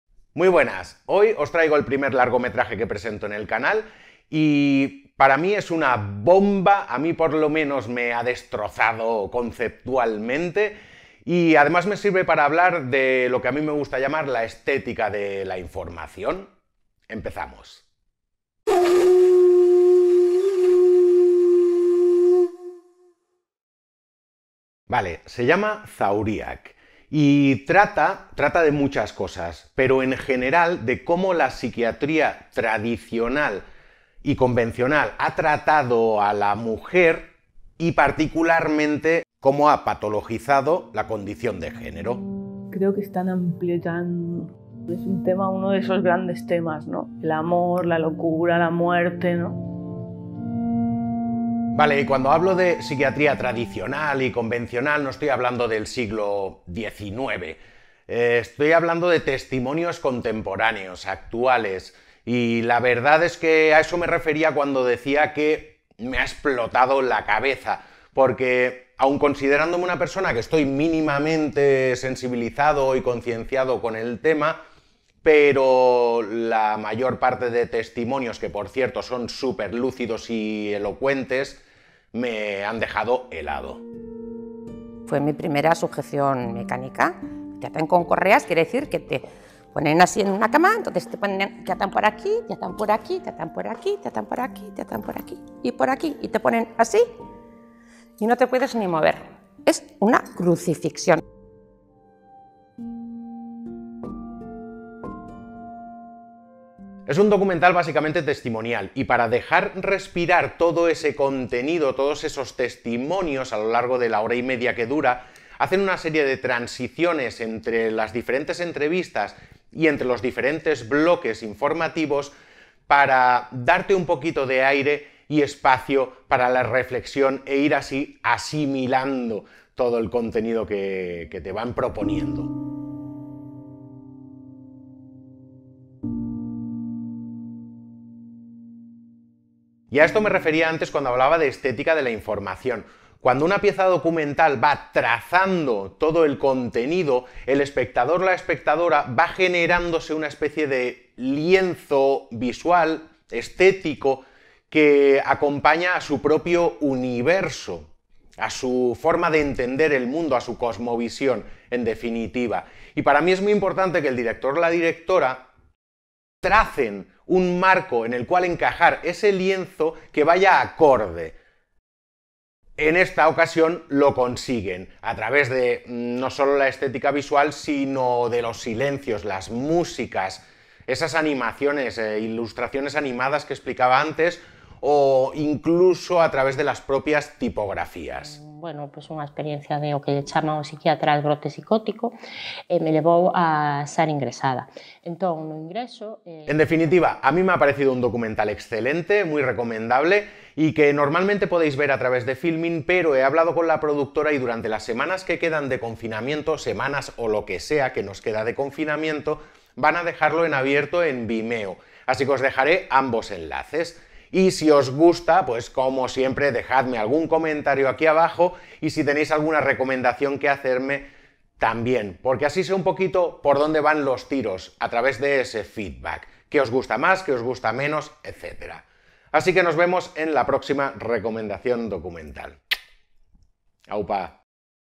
LOCURA, PSIQUIATRÍA y GÉNERO en un largometraje documental (DOCUMENTALES RECOMENDADOS #7🎬)
El primer largometraje que recomiendo en el canal. Un documental que reflexiona sobre qué es la locura y como la psiquiatría convencional ha patologizado la condición de género. Podéis verlo clickando aquí 👇 abajo!

ZAURIA(K)
Maier Irigoien Ulaiar
Iker Oiz Elgorriaga
Isabel Sáez

Enlace a Vimeo (sólo disponible durante el confinamiento) VOSE-VOSC

Que os la gocéis 😀!!!

Si queréis ver el tráiler de Theatrum🎥🎬🎭, documental en el que estoy trabajando actualmente, podéis hacerlo en 👇

Un abrazo, salud y libertad💚!